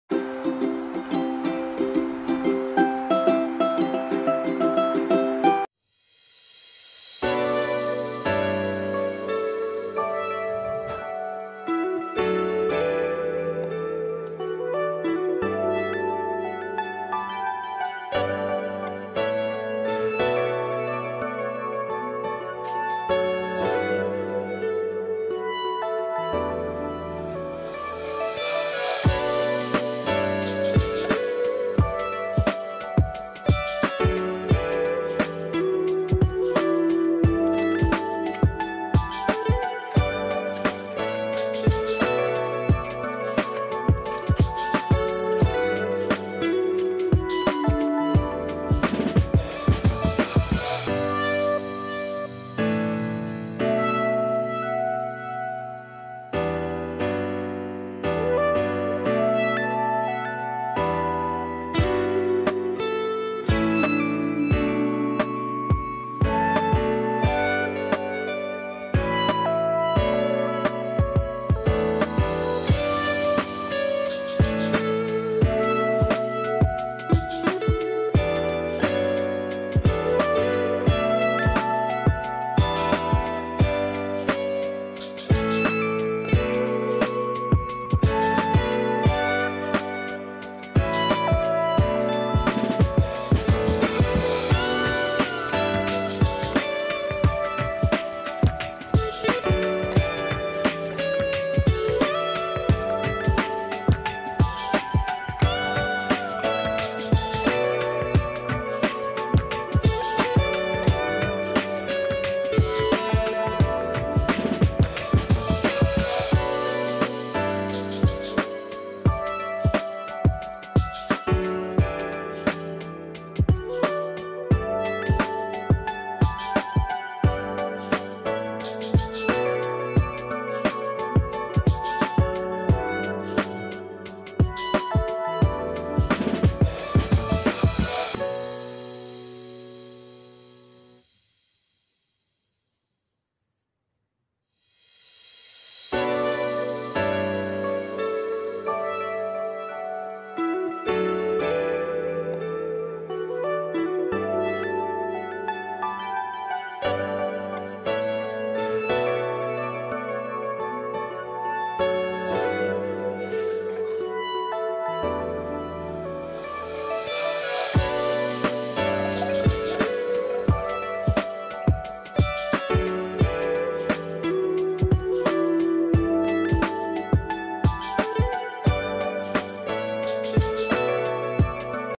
Magic Salt | How to Get Rid of Drain Flies | Home Remedy
In this video, you will learn about the control of drain flies in bathrooms,kitchen and toilet etc with home remedies ( backing soda and vinegar ).